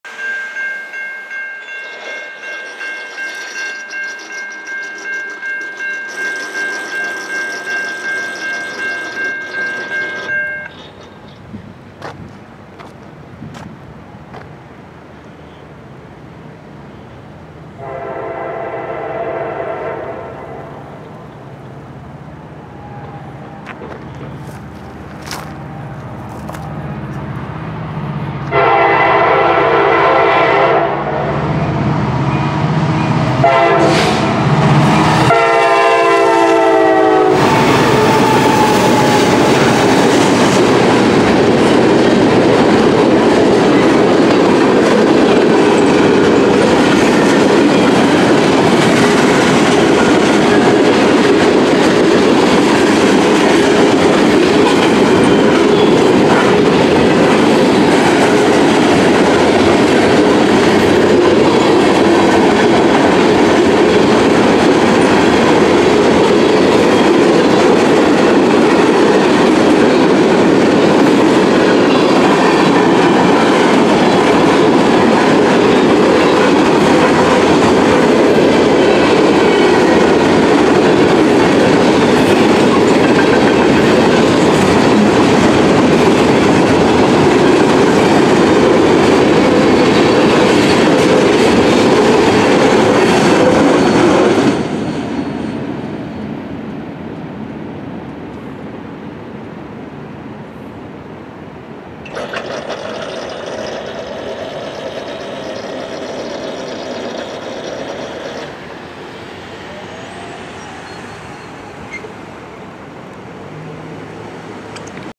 Olive St. Grade Crossing (Elyria, OH) 10/20/17
This crossing is located on the NS Chicago Line.  The main signals were installed in the late 2000s replacing Conrail signals.  The pedestrian gates were carried over from the Conrail installation.

Northern Signal
Bell:  None
Base:  Safetran Single Sided
Lights:  2 pairs of 12"X20" Safetran incandescents
Light Bracket:  Safetran
Gate Lights:  4" RECO incandescents
Gate Mechanism:  Safetran

Southern Signal
Bell:  Safetran Type 3 E Bell Low Tone
Light Bracket:  Safetran
Gate Mechanism:  Safetran

Northern Pedestrian Gate
Base:  WRRS Single Sided
Gate Light:  4" NEG incandescent
Gate Mechanism:  WCH

Southern Pedestrian Gate
Gate Mechanism:  WCH